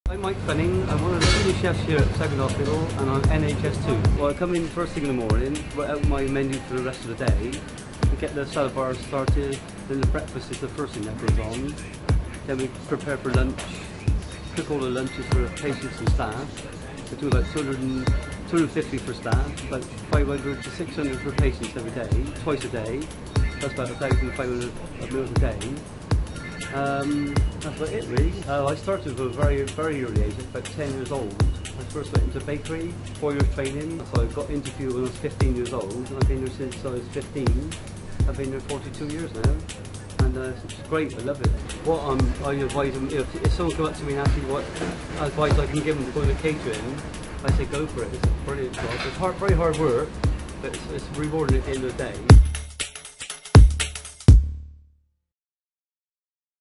Mike a Chef
Mike a Chef at Southmead Hospital talks about working in the NHS. Video produced by Elliot Lamb, Seb Swift, Jamie Carter and Katherine Shaw as part of the Creative & Media Diploma group at Backwell School